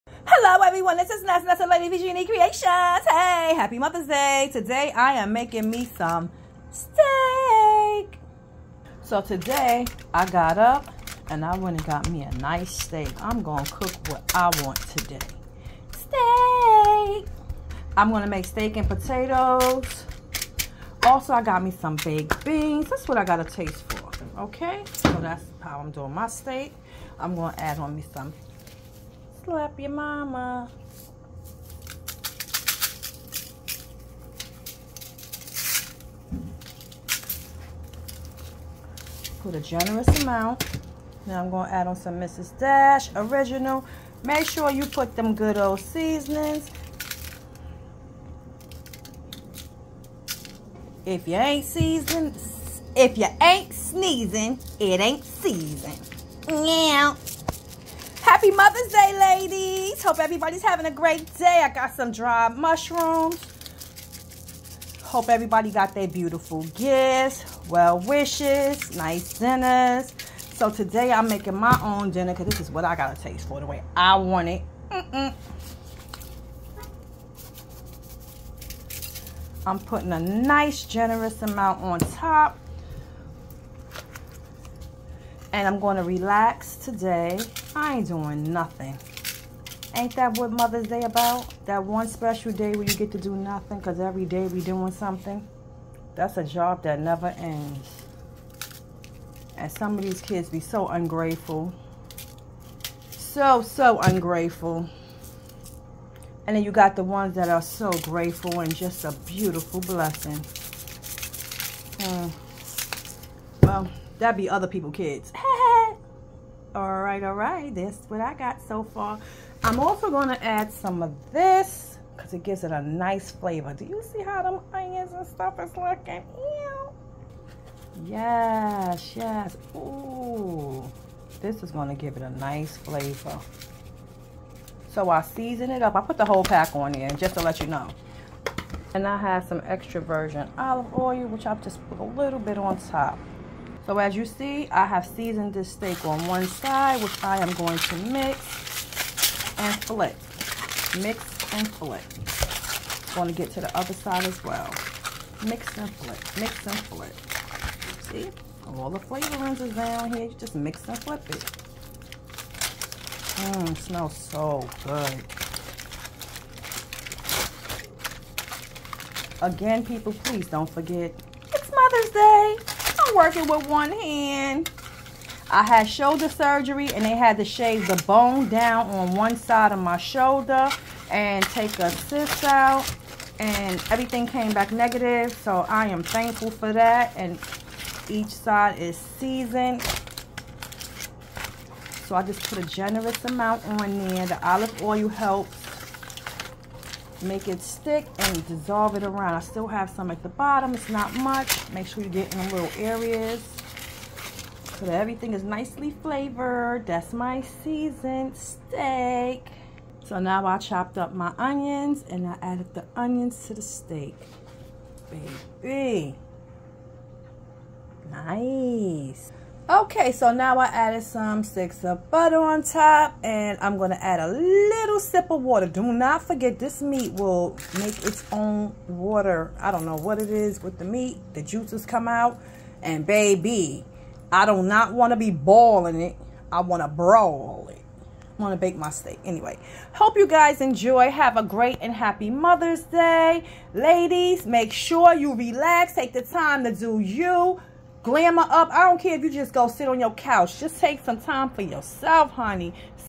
Mother's Day Collard Greens Seasonings with Smoke Hammocks
Seasoning my Mother's Day steak